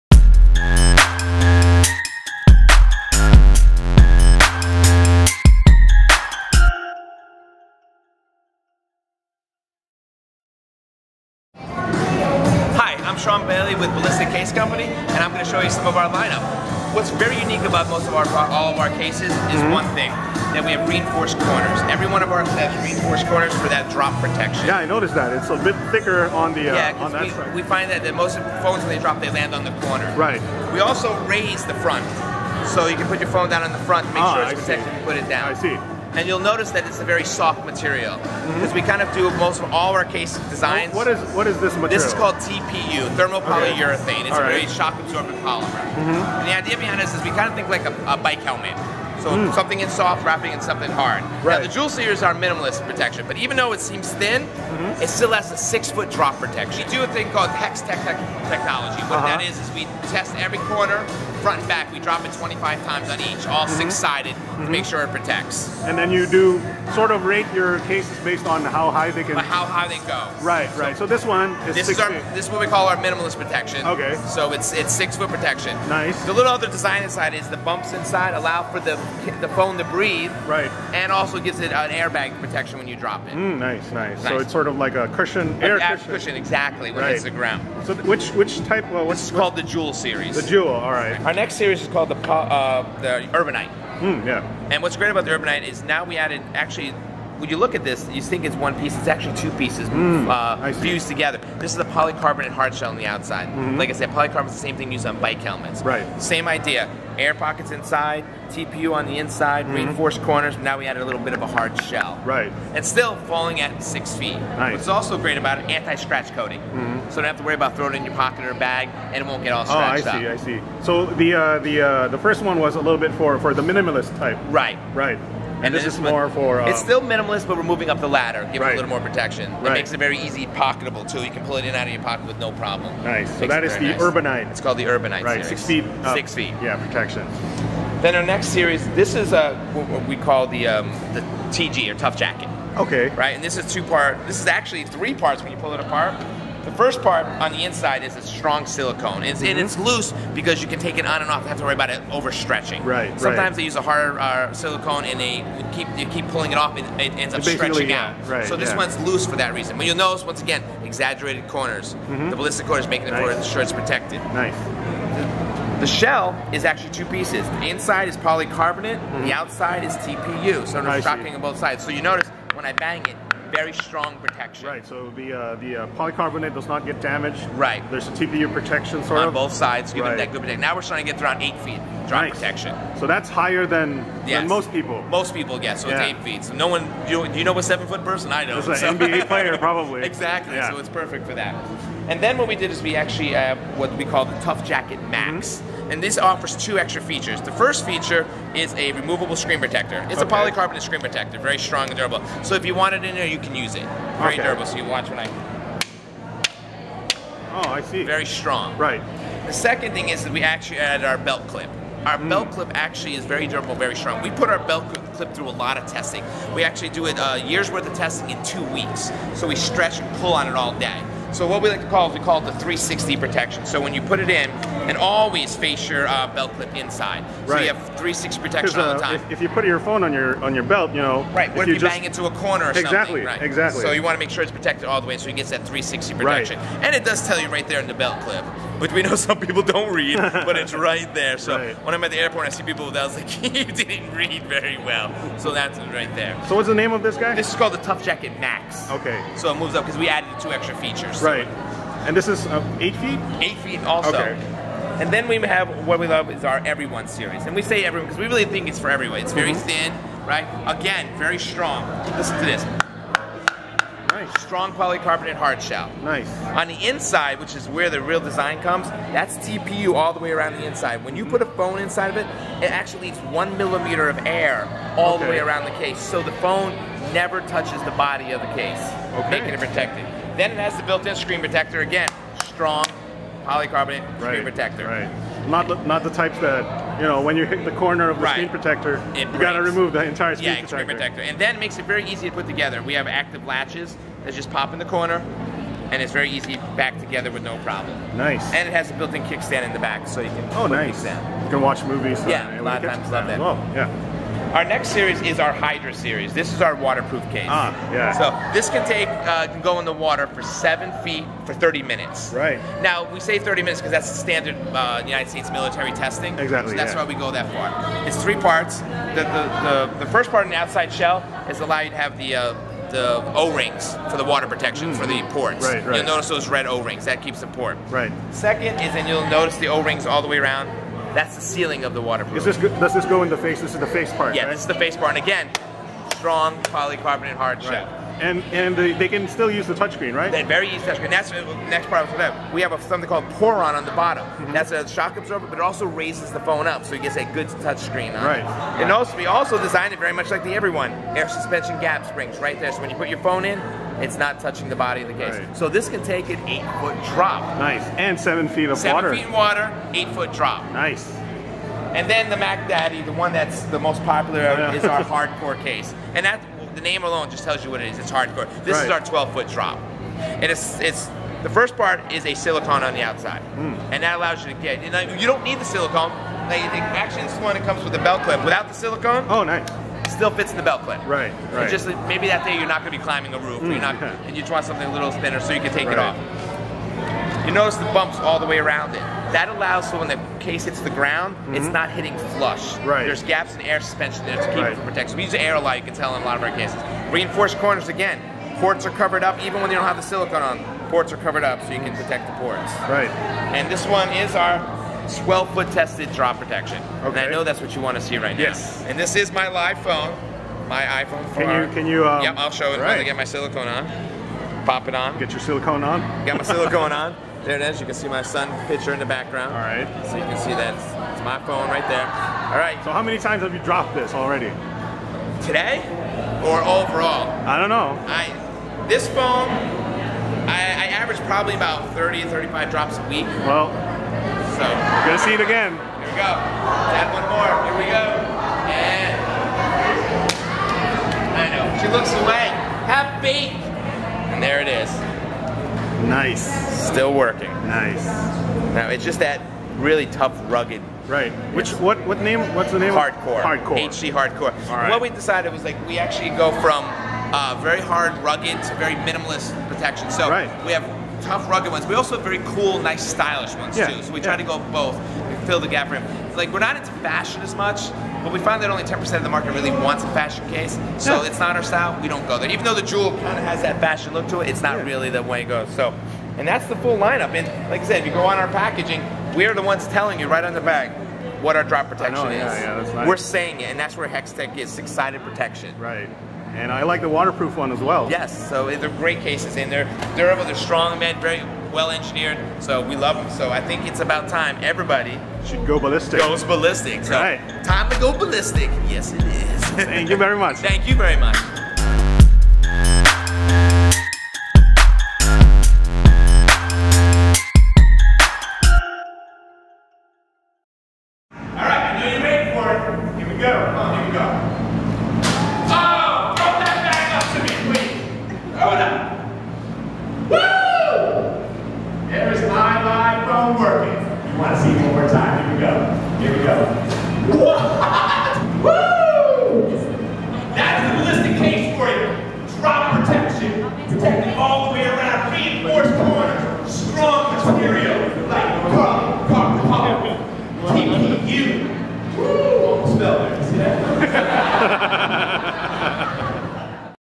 Ballistic Cases Launch Preview - Rugged Line Of Protection For iPad, iPhone & Galaxy Devices
A disruptive newcomer in the rugged smartphone case business is looking to make a grand entrance here in the Philippines in the name of the Ballistic Case brand. Aggressively calling out other brands, Ballistic makes us aware in no subtle terms that they mean business; and they're not afraid to prove it -- by dropping their phones from as high as 12 feet. With a range of cases from the minimalist to the heavy duty (and even one that's waterproof), Ballistic is offering protection for every kind of smartphone or tablet user -- and prices are fairly competitive too. Check out our video preview of each type here and see which one is for you!